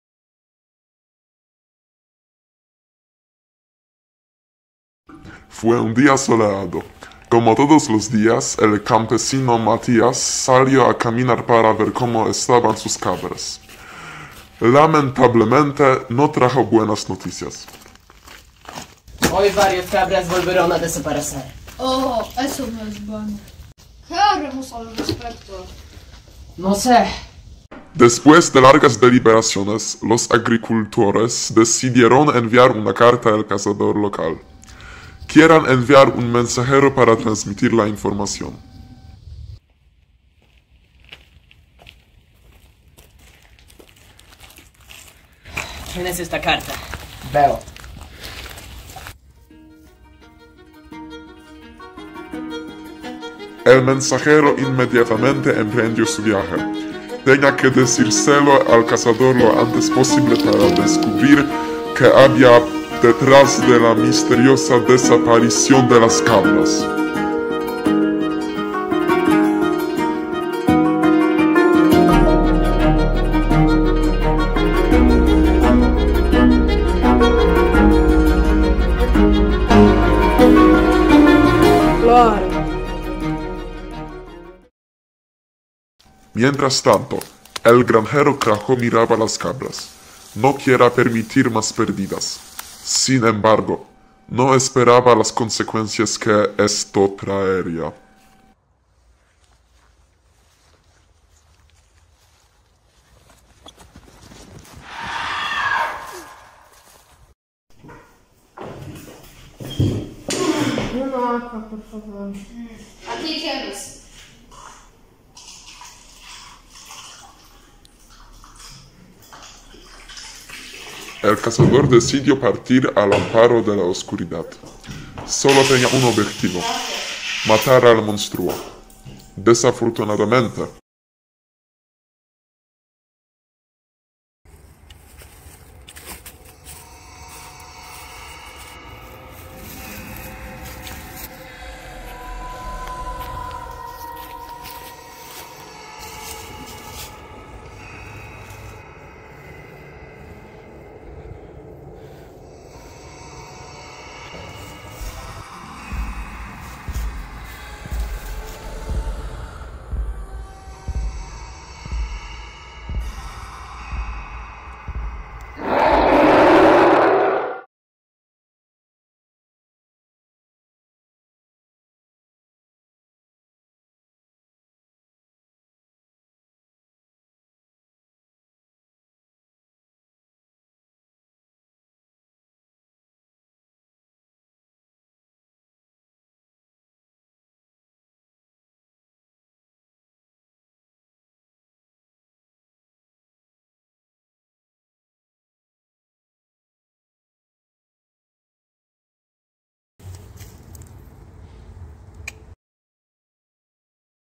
Chupacabras. El aullido de la noche
VII Concurso ELE en Corto de la Consejería de Educación en Polonia. Categoría I. Tercer premio.
Grupo: BoKKaMaJo. Niepubliczna Szkoła Podstawowa Open Future International School (Cracovia).
Alumnos: Bartosz Langman, Jan Kornecki, Kacper Ryznar, Krzysztof Olearczyk y Maciej Francuz.
Profesora: Anna Butkiewicz.